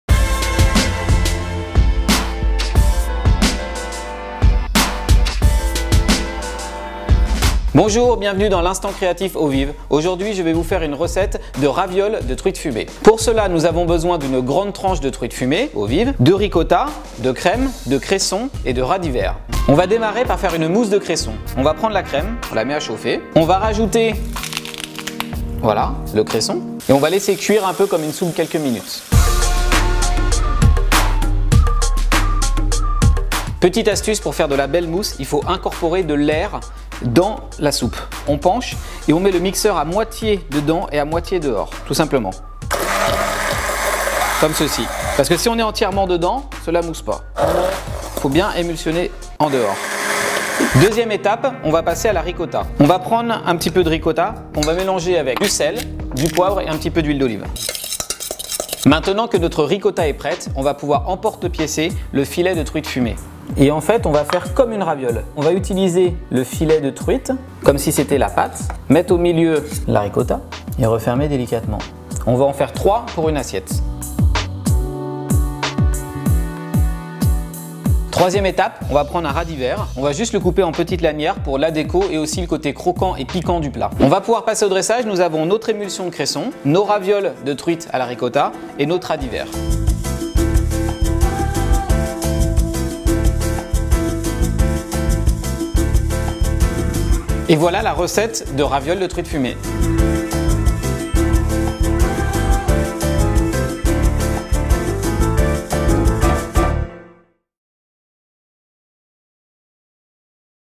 Ravioles de truite fumée par Jean Imbert
Grâce aux instants créatifs de Jean Imbert, découvrez la recette des ravioles de truite fumée